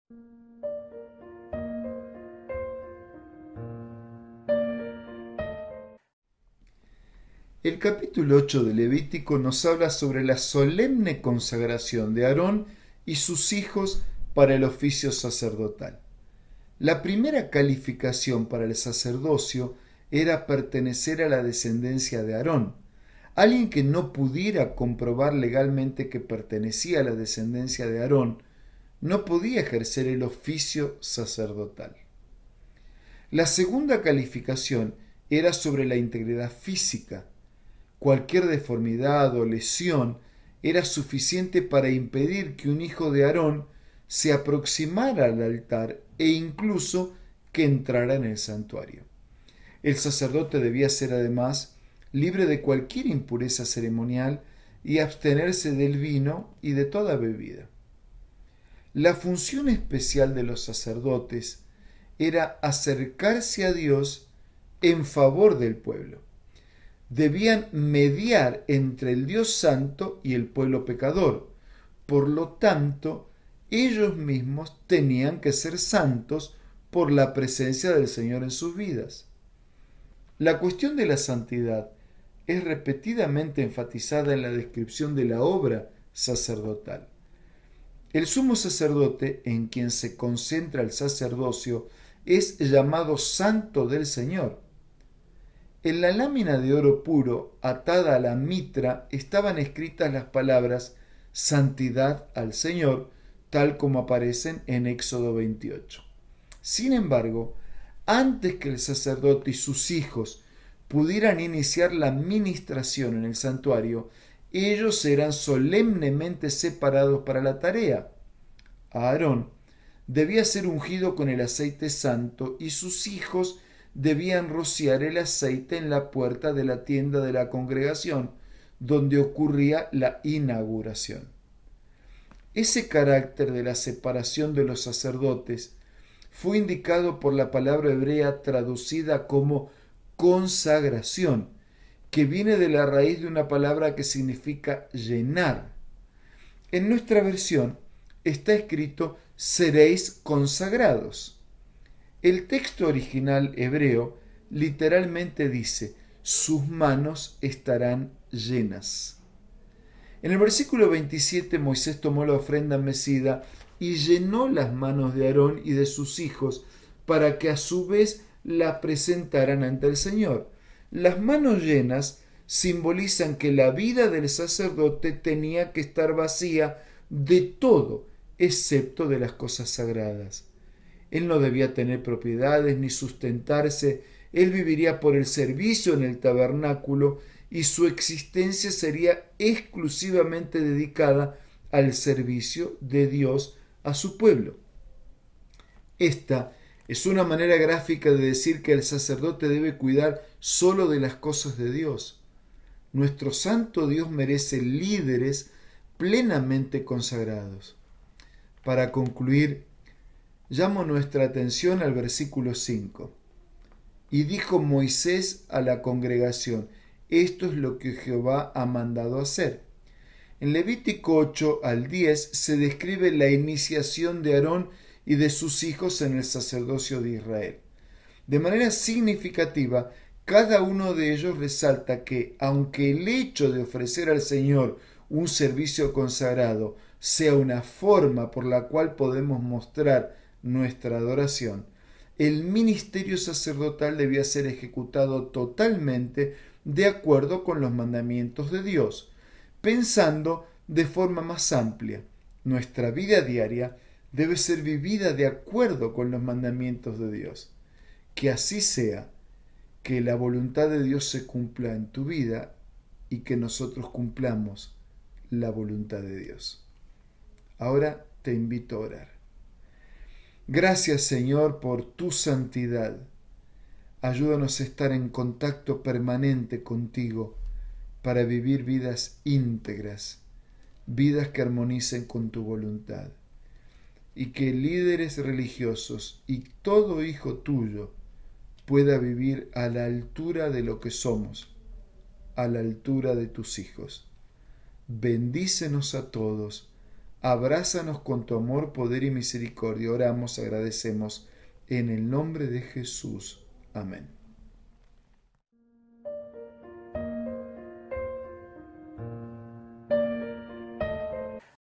Levítico 8 - Reavivados Por Su Palabra
Este capítulo nos habla sobre la solemne consagración de Aarón y sus hijos para el oficio sacerdotal. La función especial de los sacerdotes era acercarse a Dios en favor del pueblo.  Debía mediar entre el Dios Santo y el pueblo pecador; por lo tanto, ellos mismos debían ser santos. Nuestro Santo Dios merece líderes plenamente consagrados. De manera significativa, cada uno de ellos resalta que, aunque el hecho de ofrecer al Señor un servicio consagrado sea una forma por la cual podemos mostrar nuestra adoración, el ministerio sacerdotal debía ser ejecutado totalmente de acuerdo con los mandamientos de Dios. Pensando de forma más amplia, nuestra vida diaria debe ser vivida de acuerdo con los mandamientos de Dios. Esta es la voluntad del Señor.